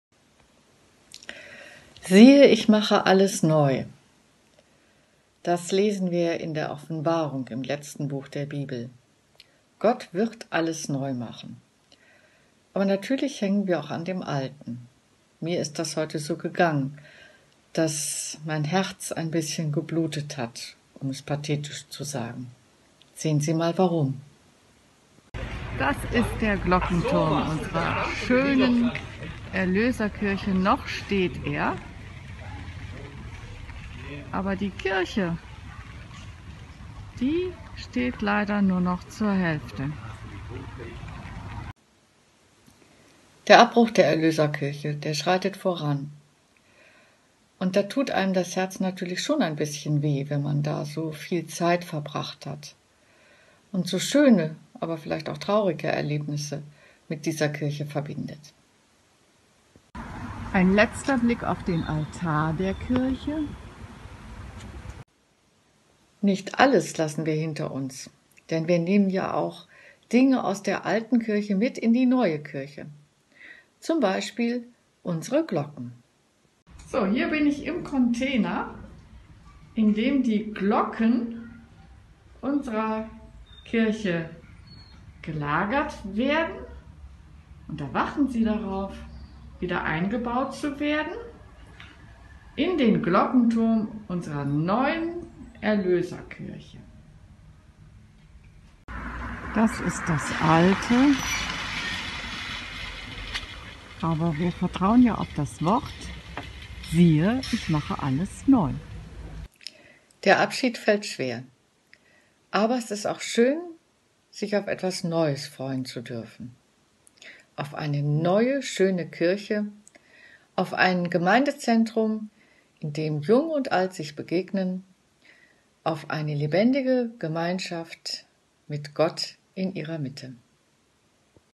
2020 07 15 MittWORT 016 | Siehe ich mache alles neu
Mit einem weinenden und einem lachenden Auge sieht Pfarrerin Schlarp auf den Abbruch der alten Erlöserkirche, der nun begonnen hat.